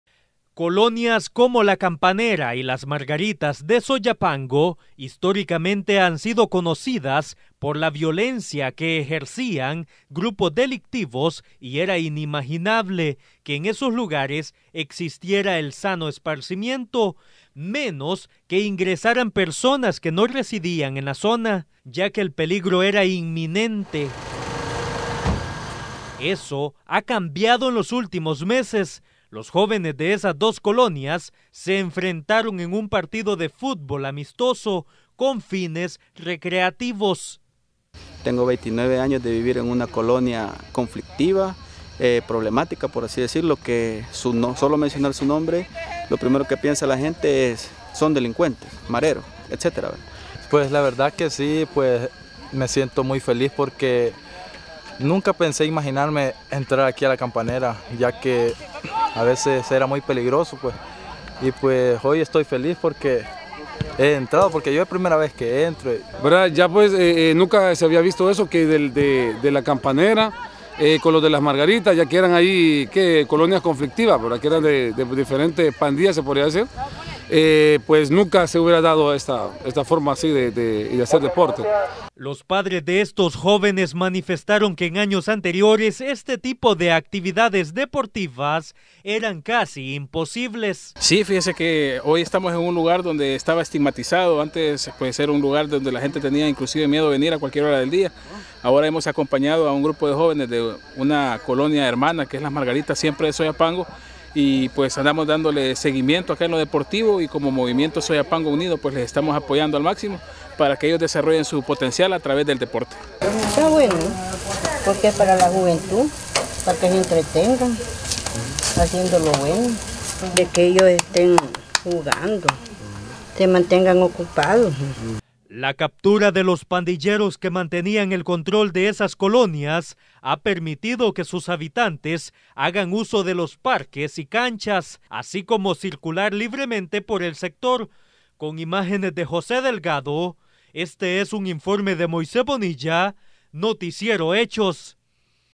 Encuentro futbolístico Campanera y Las Margaritas
Jóvenes de dos colonias de Soyapango que eran conocidas por alto índice delincuencial, disputaron un partido de futbol amistoso.